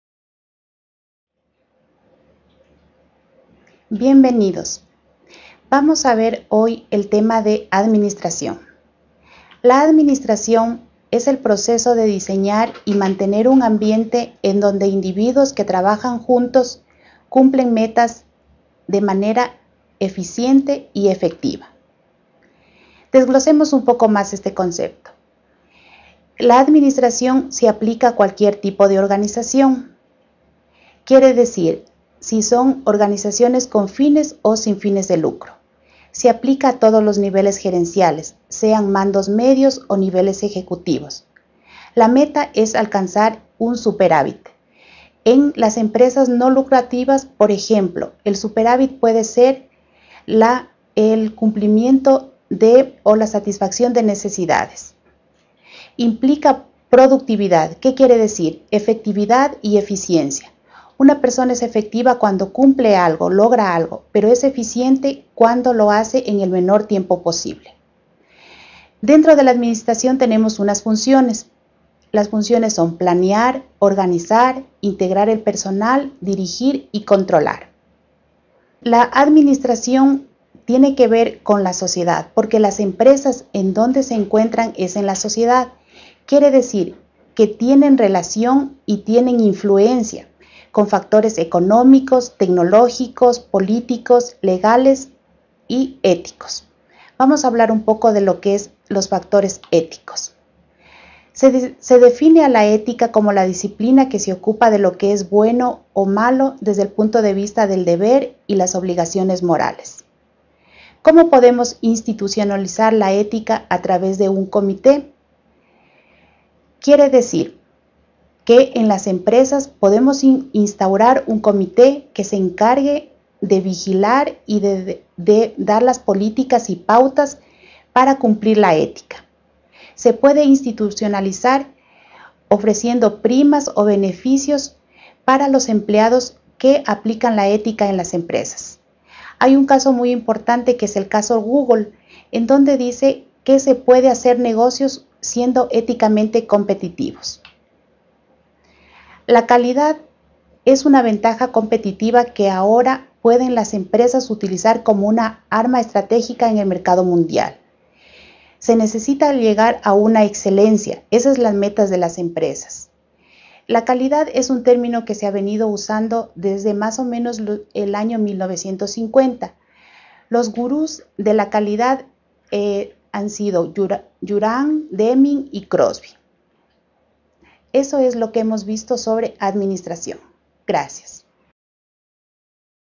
UTPL ADMINISTRACIÓN [(INFORMÁTICA)(ORGANIZACIÓN Y ADMINISTRACIÓN EMPRESARIAL)]
Informática
Organización y Administración Empresarial
Tema: Administración
Ponente: Lic. María Verónica López